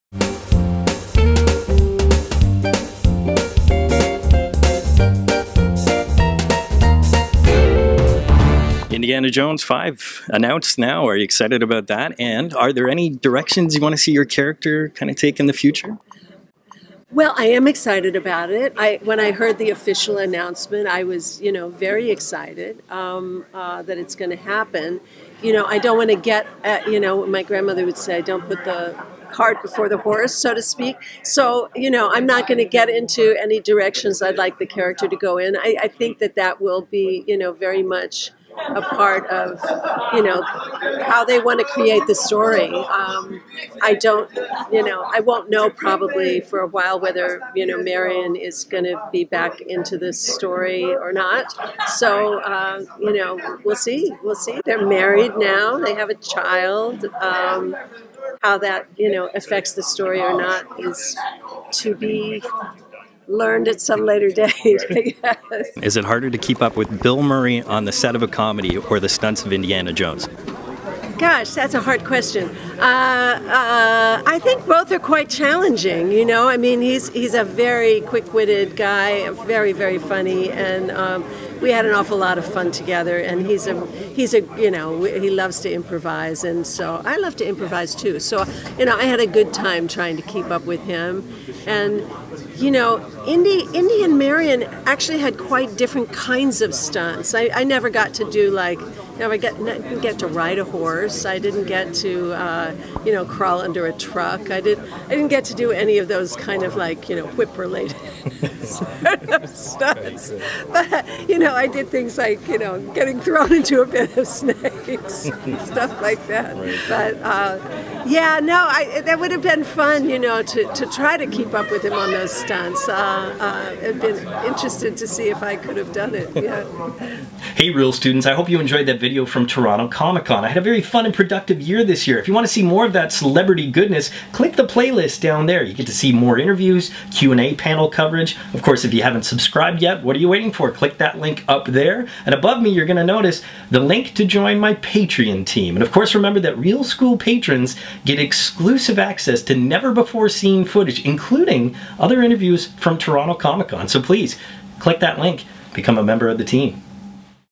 KAREN ALLEN talks INDIANA JONES 5!
At Toronto Comic Con, the lovely Karen Allen discusses the future possibilities for Marion in the Indiana Jones sequel, as well as a couple of her famous costars, Harrison Ford and Bill Murray.